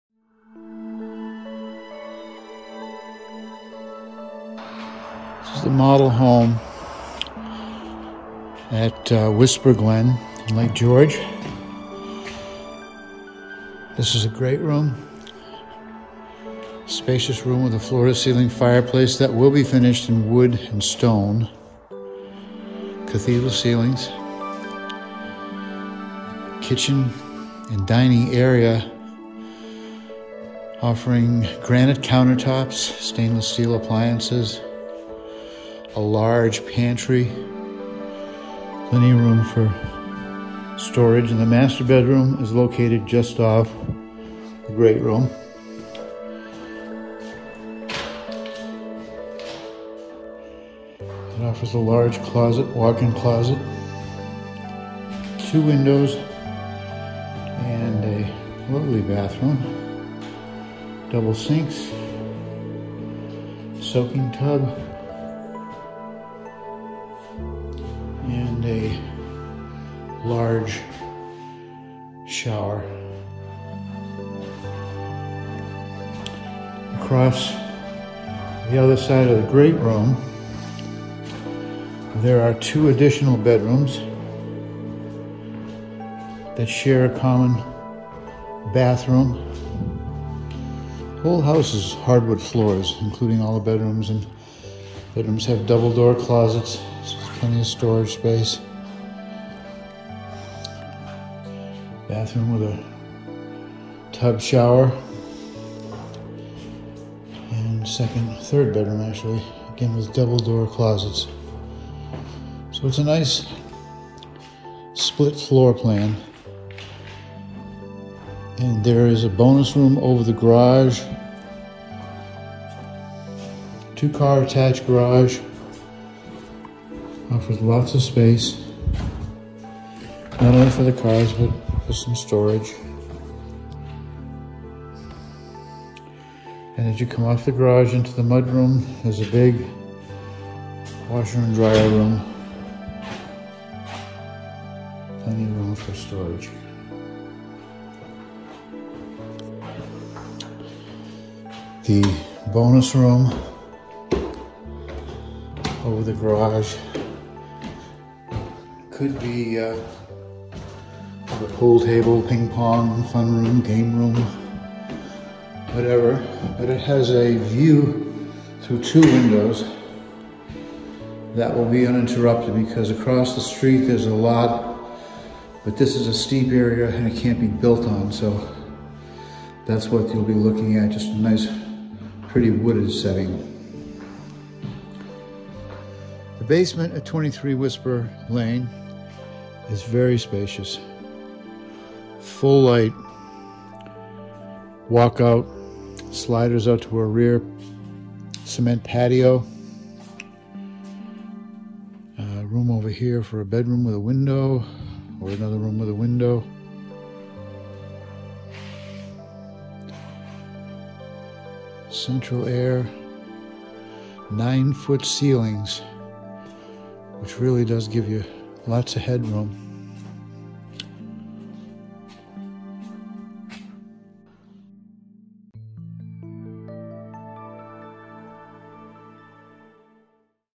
23 Whisper Lane, Lake George, NY
This new home is nearing completion and offers several outstanding features. The neighborhood is very convenient to both the Northway and the lake with three new homes under construction on this small cul-de-sac. This home has a very large walk out basement with high ceilings which is unfinished but provides excellent expansion for the potential living space.